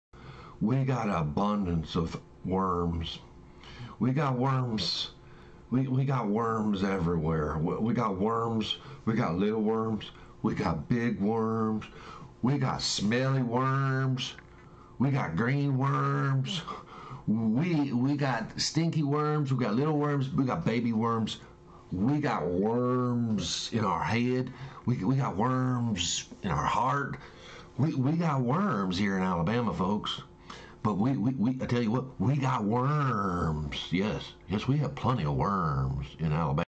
Scott77 Has Worms
Scott77 talking about the worms in his life.